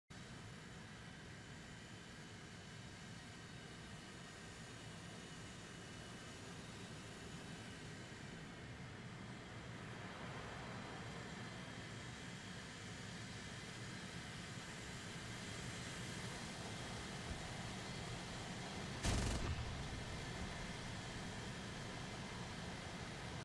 Cannon buff LUL
Ok seriously, cannons are much improved this patch. But damn, this still happens to me too often.